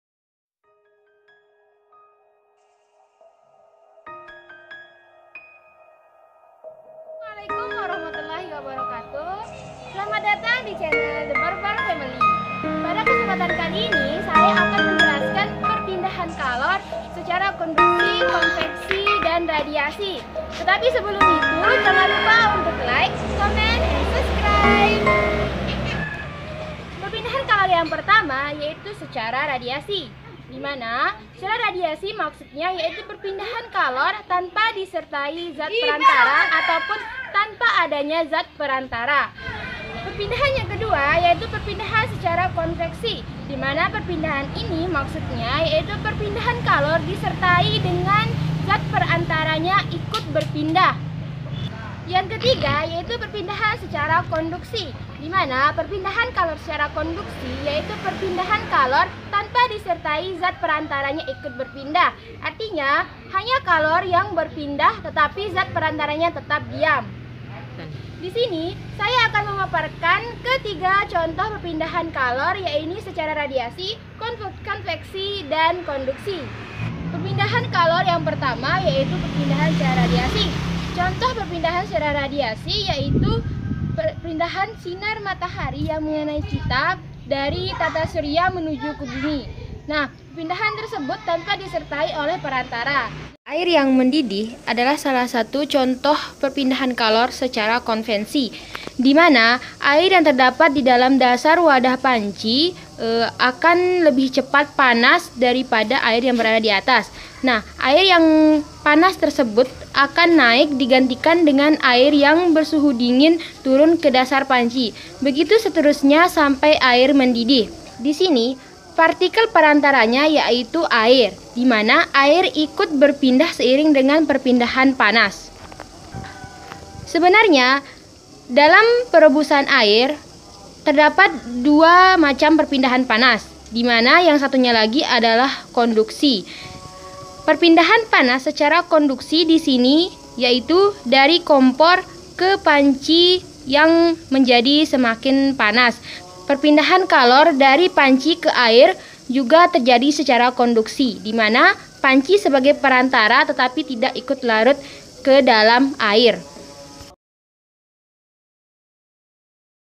Perpindahan Kalor secara konduksi, konveksi, dan radiasi by Hulwatul Faizati
Pelajaran fisika kelas 10 Materi tentang perpindahan kalor (konduksi, konveksi, & radiasi) Pada video ini dijelaskan perpindahan kalor dalam kehidupan sehari-hari